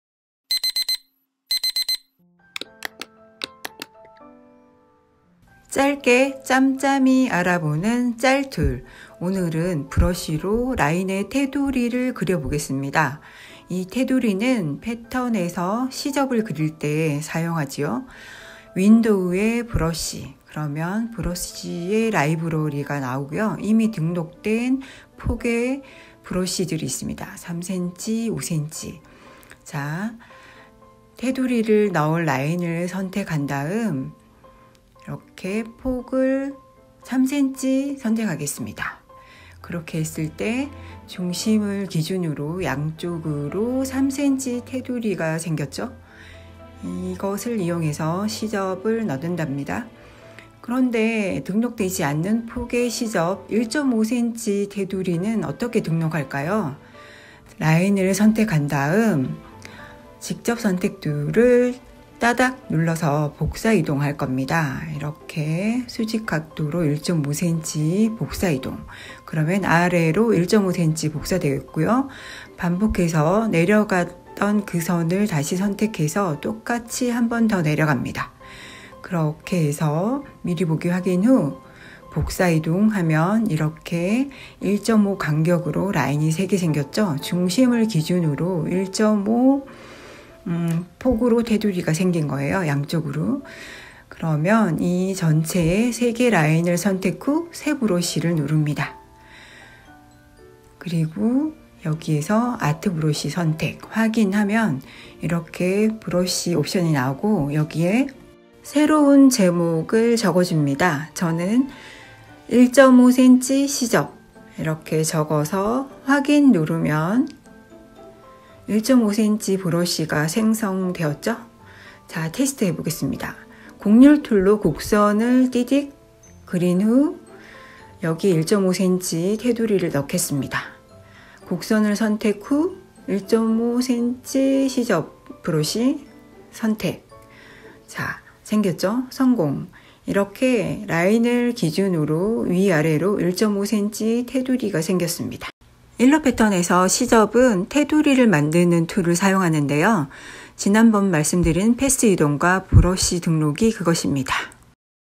일러 짧tool-브러쉬로 테두리_59
일러패턴에서 시접을 넣을 때
사용하는 브러쉬 기능입니다!

그림은 그릴 때 부러쉬 두께설정을 하는데
우리는 패턴에서 시접의 폭이라고 생각하고 사용하죠~

시접은
패스이동과 브러쉬
두가지 기능으로 충분히 만들 수 있어요^^

오늘은 안방에서 그냥 아이폰으로 녹음했더니
울림이 좀 있군요 ㅠㅠ
일관성 없는 유투브 작업공간 ㅋㅋㅋㅋㅋ

아무튼

다음
12강에서 시접패턴 길게 보여드릴게요~*